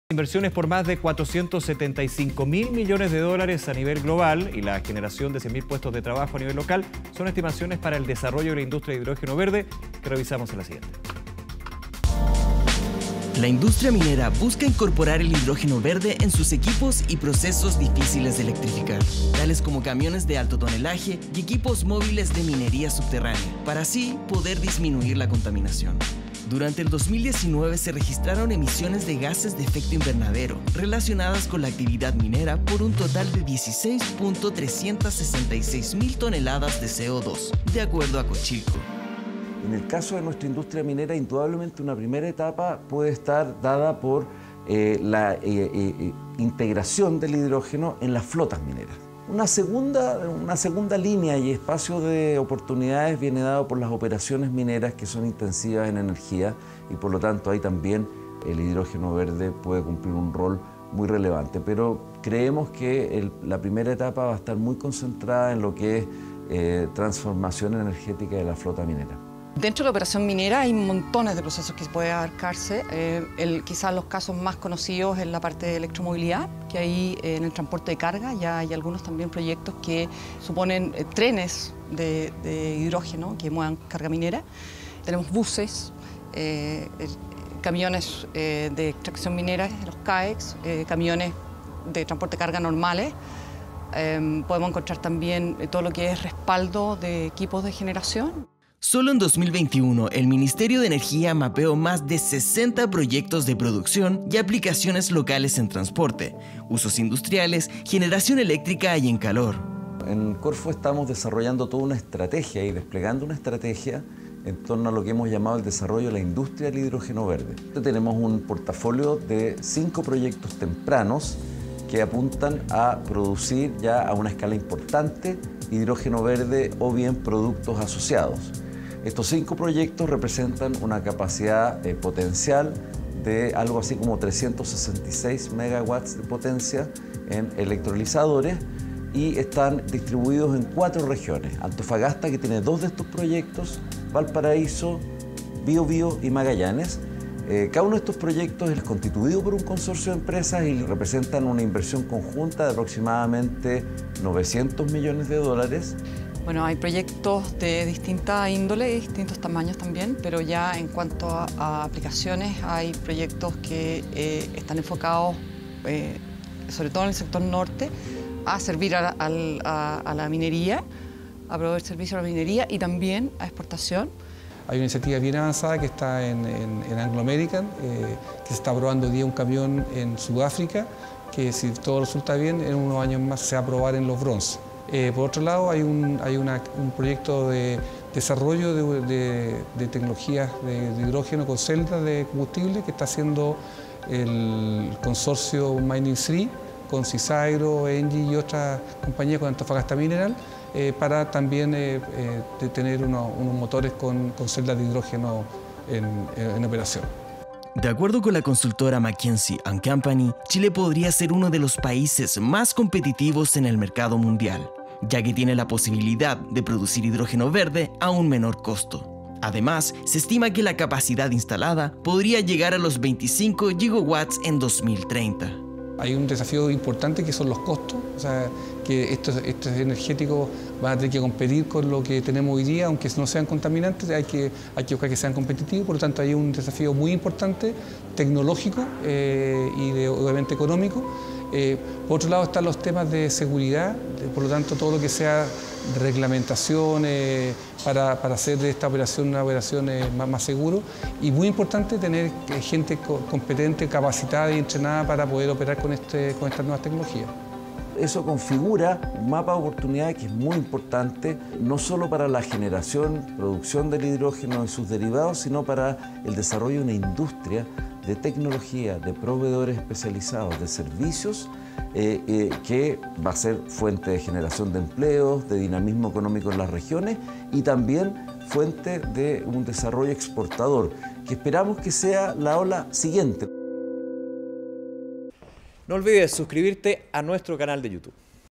Hidrógeno verde en la cadena de valor minera
Inversiones por más de US$475.000 millones a nivel global y la generación de más de 100.000 puestos de trabajo a nivel local, son las estimaciones para el desarrollo de la industria del hidrógeno verde que se proyecta como un combustible limpio, que permitirá disminuir la huella de carbono y crear una nueva industria en el país.